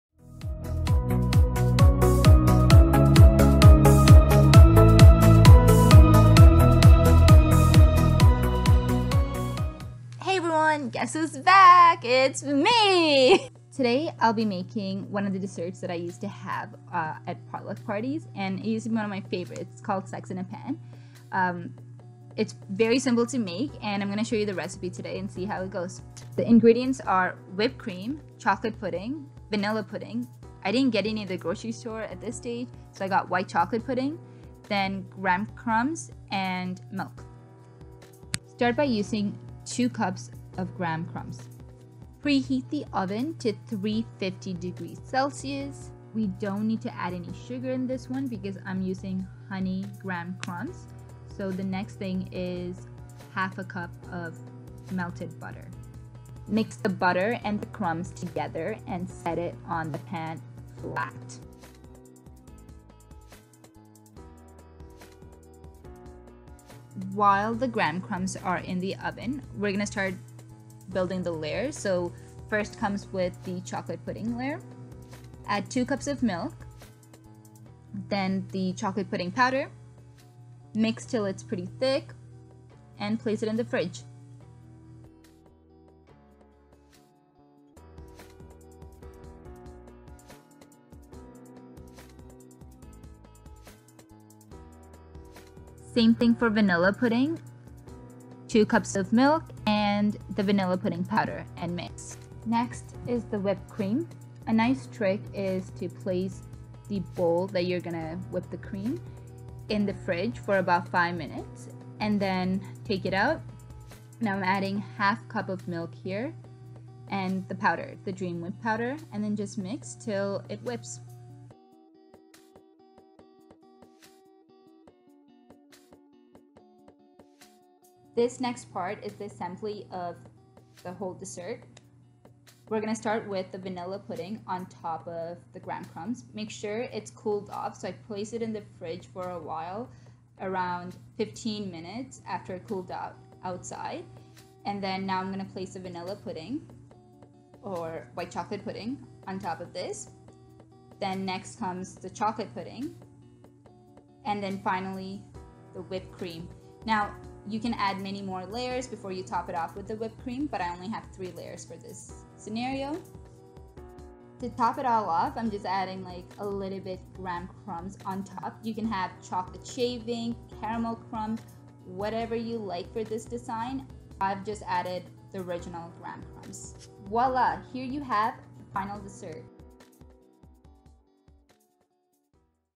S*x in a pan | MUST TRY RECIPE!!!
One of my favorite desserts I would look forward to at Potluck parties in Toronto. Easy dessert to make and tastier treat :). Just a bit modified than the original one. If you would like you can add cream cheese as a layer too.